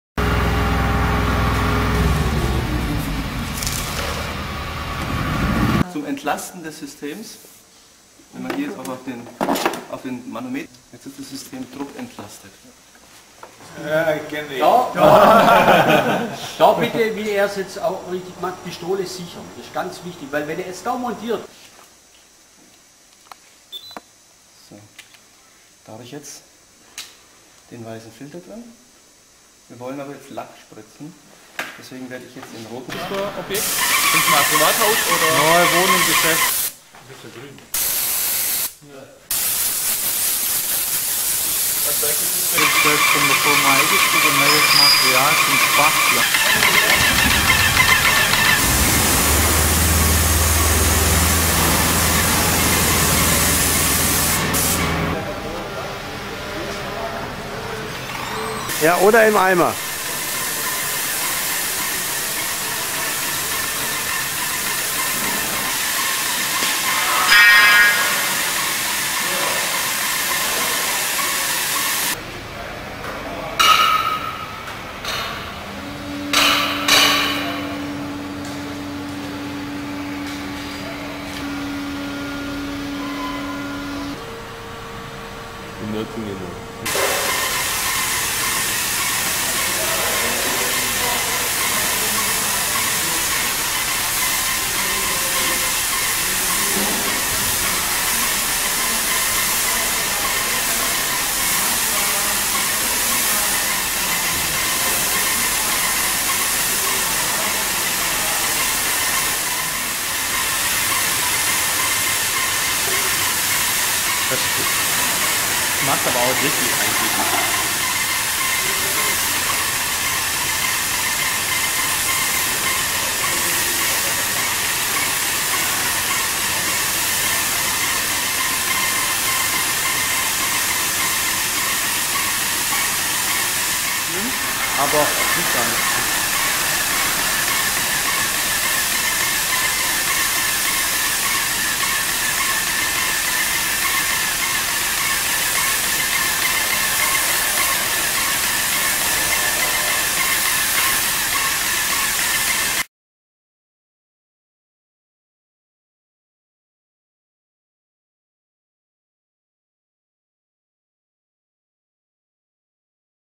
Putzmeister A 190 Airless.mp4
Putzmeister airless A190 bij een muurverf project.